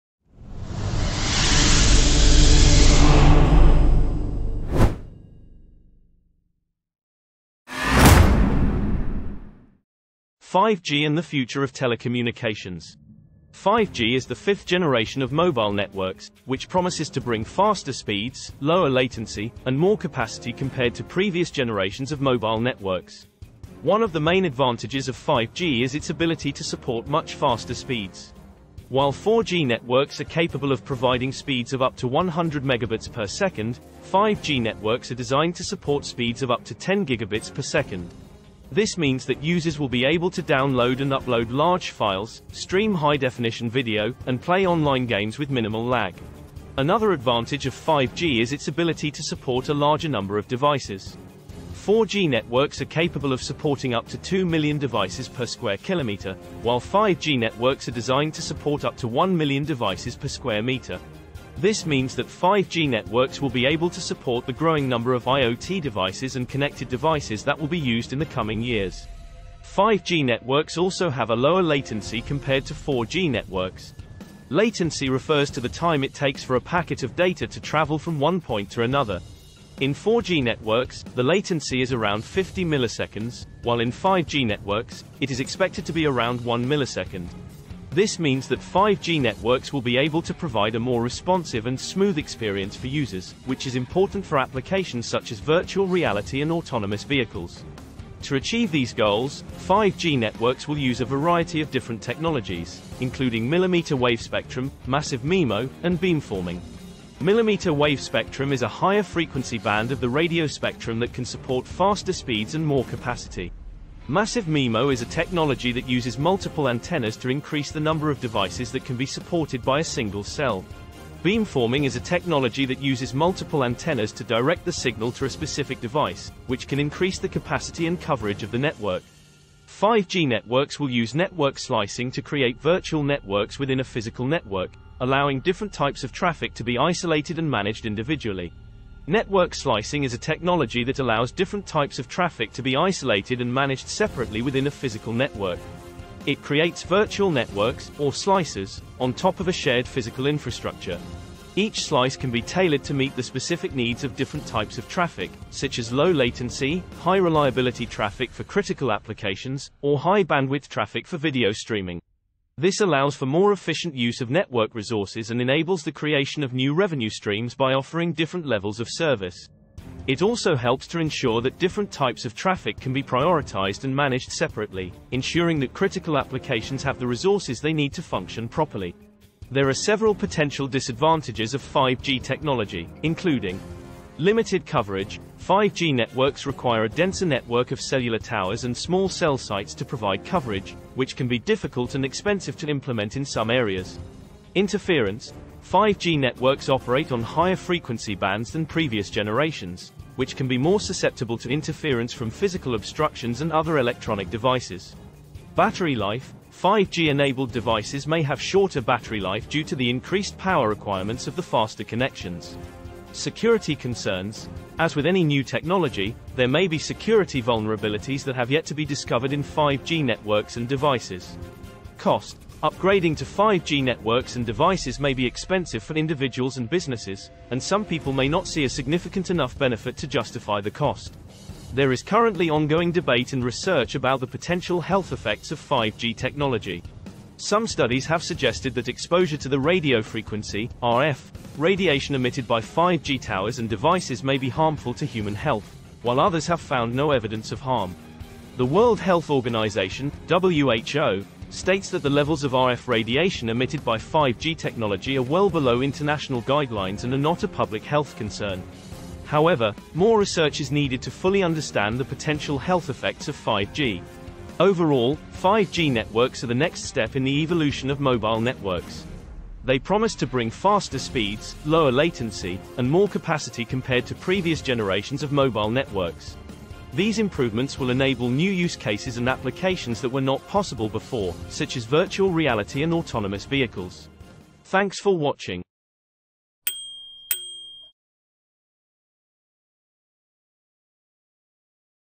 Is 5G The Future Of Telecommunications
Learn about the future of technology with our 5G educational video! Discover what 5G is and how it differs from previous generations of mobile networks. Explore the potential benefits of 5G, such as faster internet speeds and improved connectivity. We'll also discuss the disadvantages of 5G. Join us as we take a deep dive into the world of 5G and discover how it will shape our future.

These resource links are in partner with our network.

Artwork For Sale - DimensionalArtwork.Etsy.Com